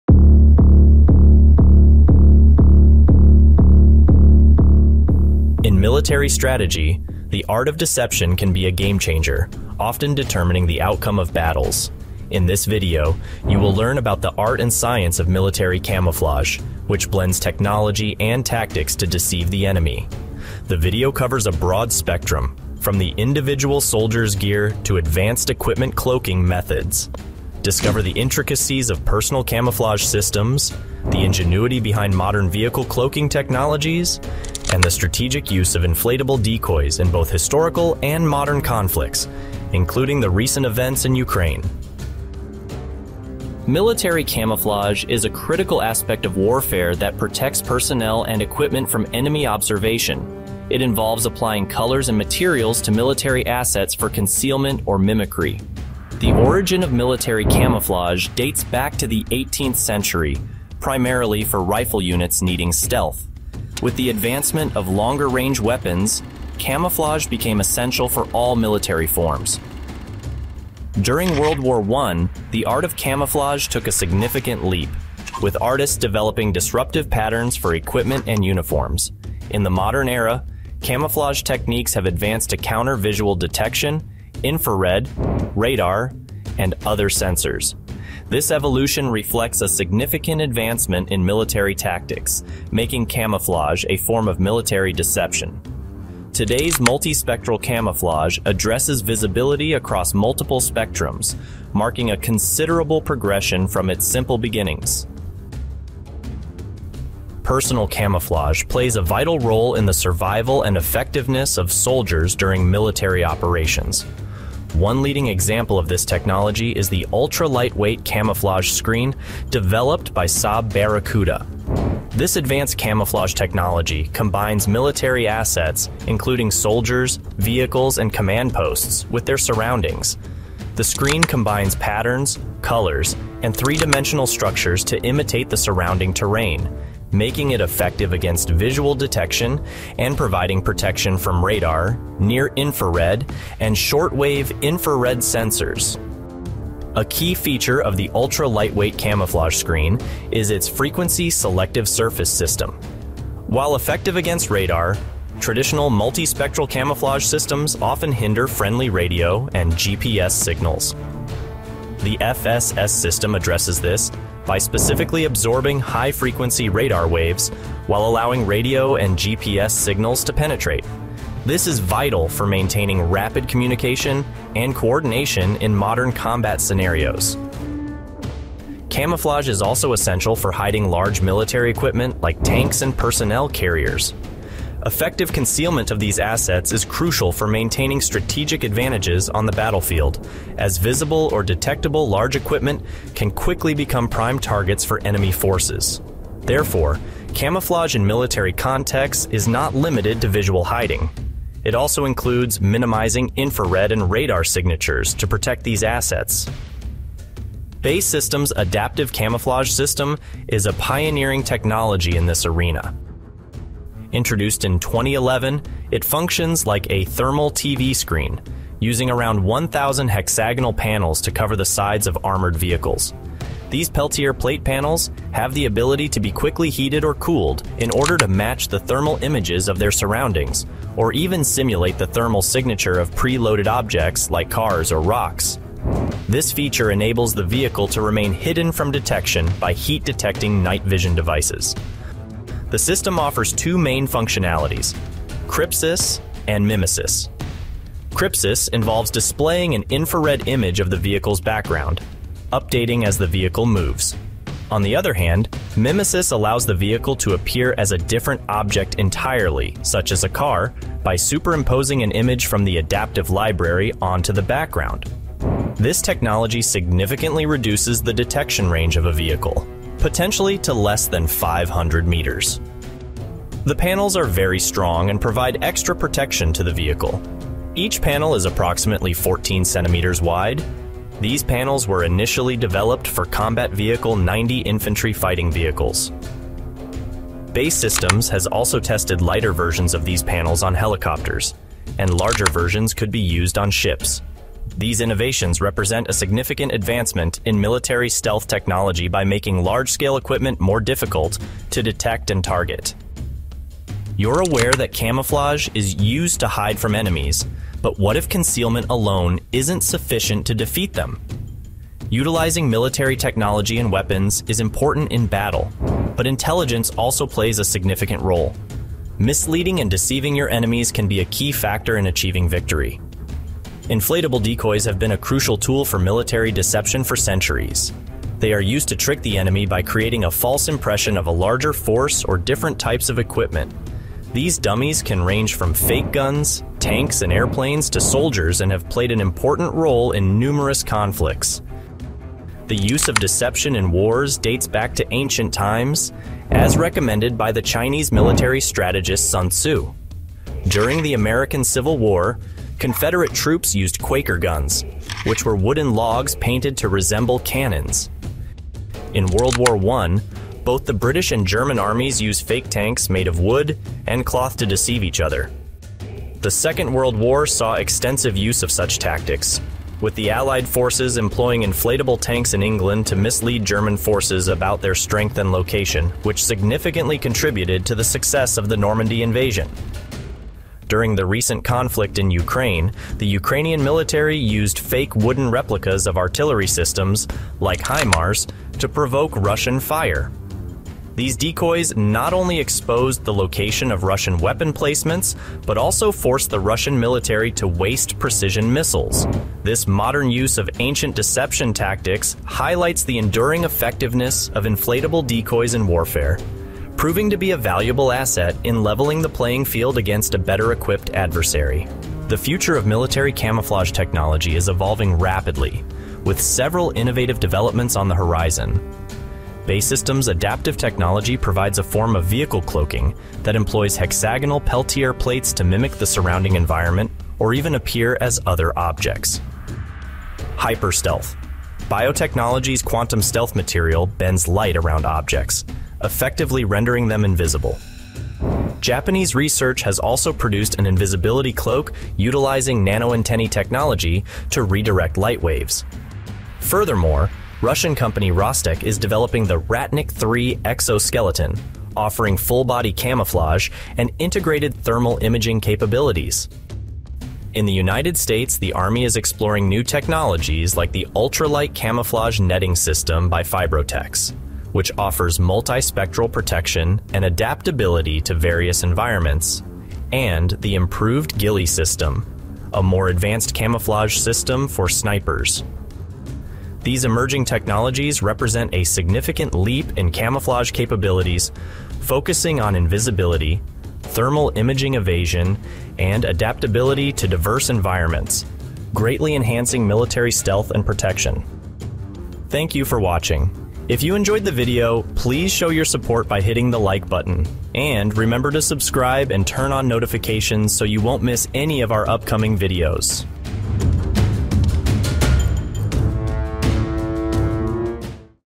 The Art of Deception: Advanced Military Camouflage
From personal gear to large-scale equipment, discover how armies around the globe use advanced methods to blend into their surroundings and gain a strategic advantage. Learn about the latest innovations in stealth technology, including the use of materials and designs that deceive both the eye and modern detection systems. Join us as we uncover the secrets of military deception and the critical role it plays in modern warfare.

⚠️ Please do NOT download or redistribute the content found on this channel.